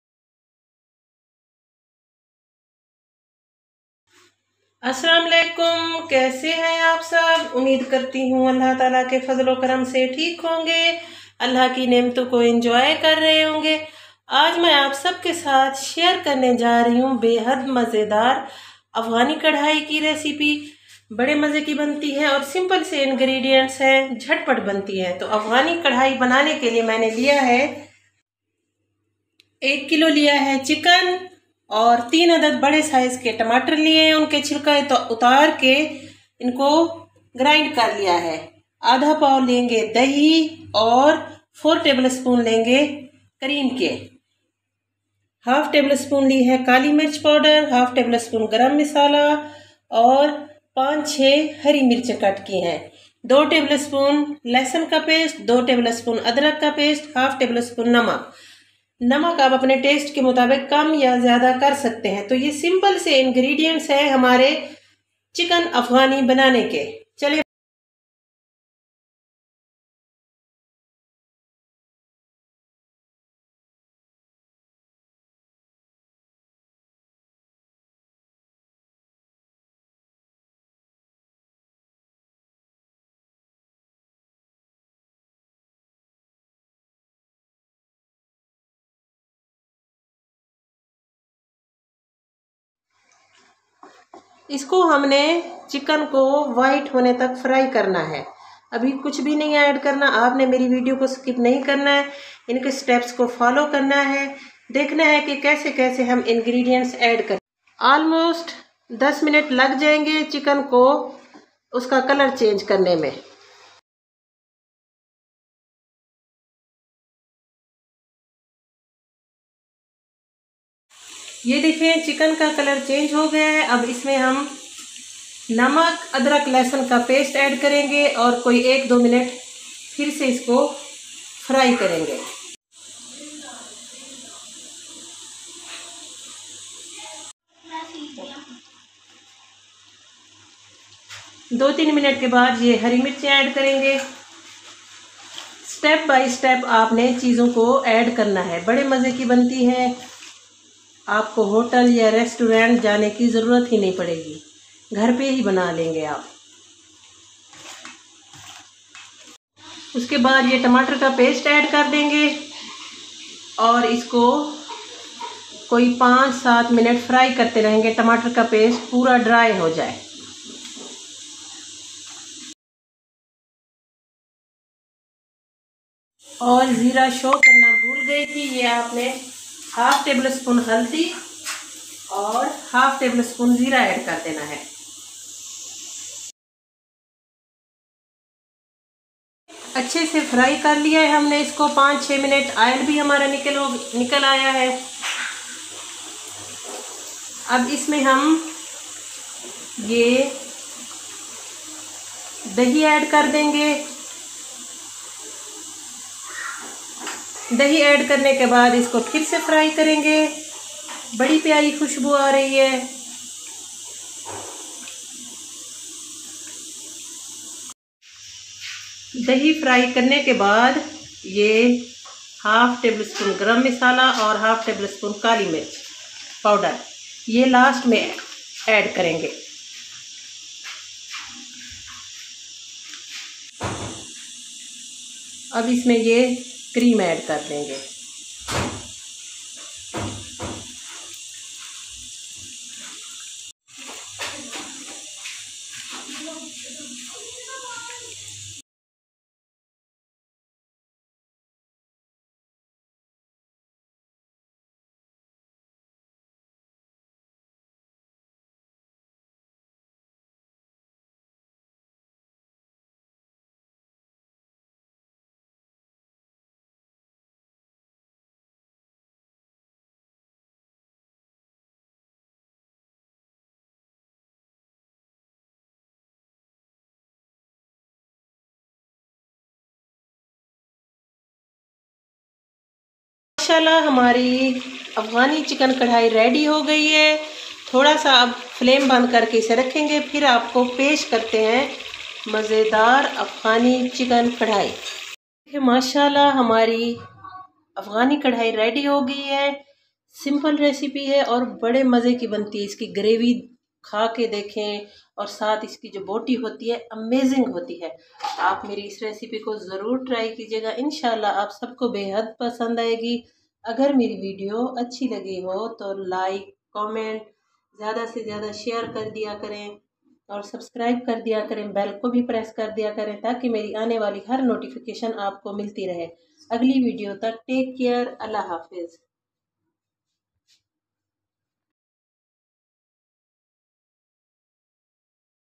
Afghani Chicken Karahi | Recipe by imrana
Afghani Karahi - a delicious Karahi recipe from Afghanistan

Afghani Chicken Karahi recipe| Recipe by imrana

Delicious and mouthwatering recipe| Quick and easy recipe | Ten minutes recipe | Recipe by imrana

Ingredients

1. Chicken 1kg
2. 3 large size tomatoes add tomato paste.

3. Cream 4 tbsp
4.Add yougurt 125 gram
5. Garlic paste 2tbsp
6. Ginger paste 2tbsp
7. Green Chilli 6 tbsp
8. Black pepper half tbsp
9. Gram masalah half tbsp
10. Zeera Half tbsp
11. Hldi half tbsp
12. Salt half tbsp... or salt to taste